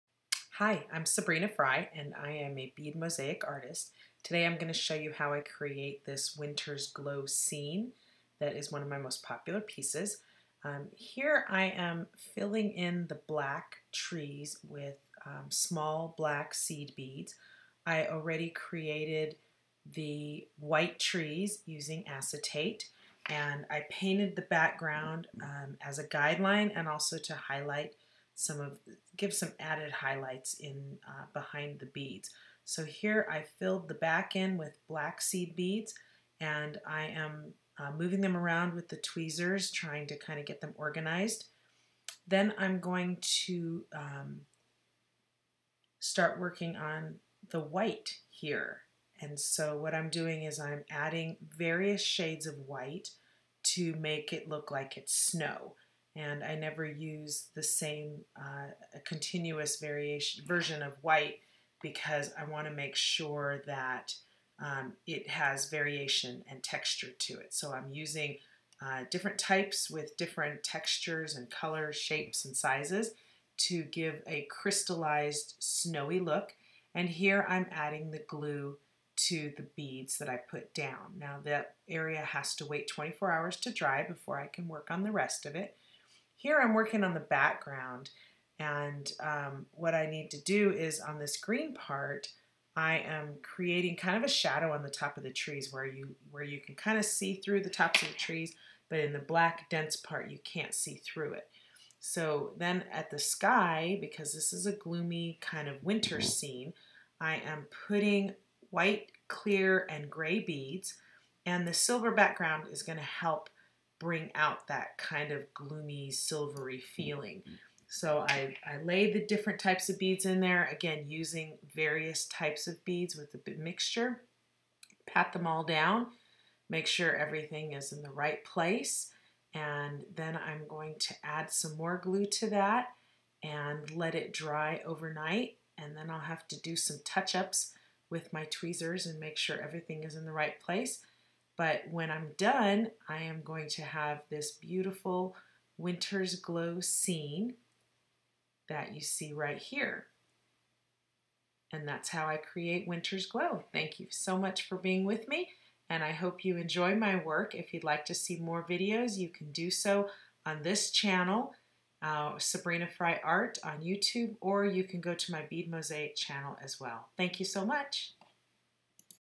You’ll fall in love with Winters Glow, watch me use seed beads to create this winter wonderland.
Seed beads are good for so much more than jewelry, Here I will create a winter wonderland using seed beads for a misty background effect.

Sabrina Frey, a professional bead artist shows you how she creates beautiful works of art using beads in unusual ways.

 Community:
join other bead lovers using their bead collections in fun and exciting ways.